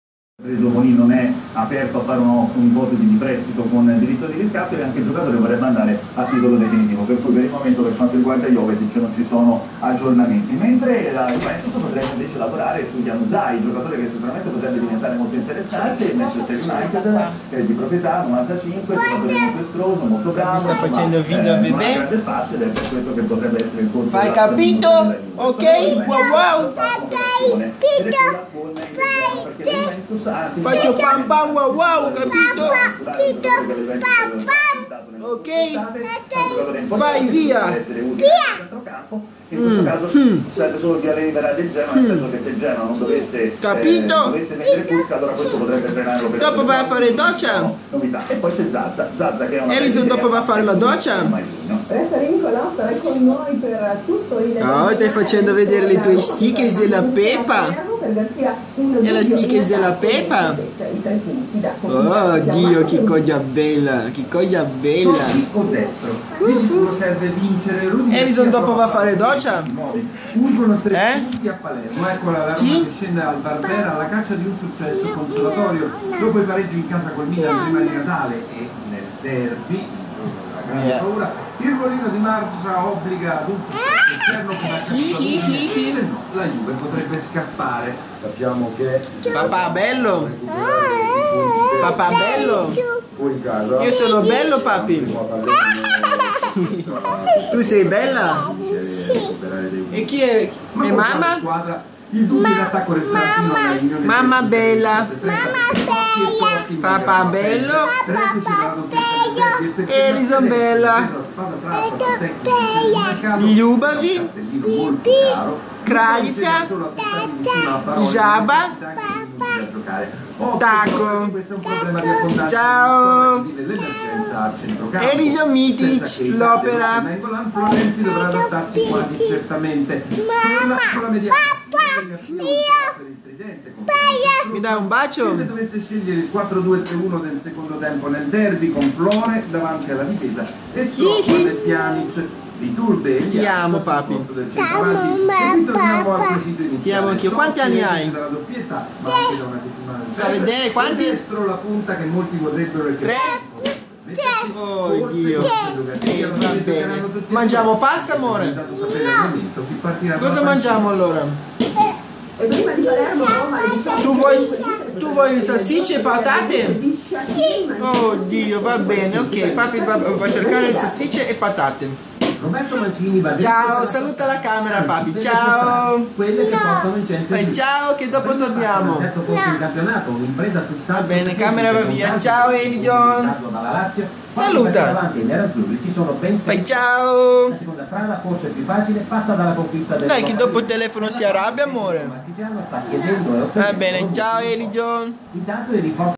My baby ALLISON MITIC LOPERA
La cosa piu bella del mondo la.mia piccola bambina di 3 anni TI AMO ALLISON